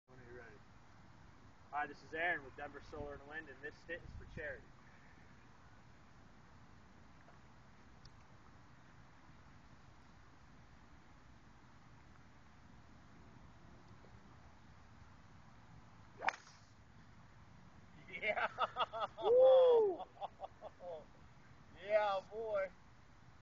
CCI & JFR Charity Golf Tournament - Denver Solar and Wind
DenverSAW at the CCI & JFR Charity Golf Tournament benefiting the Rocky Mountain Childhood Cancer Foundation. Can't wait to see you guys next year!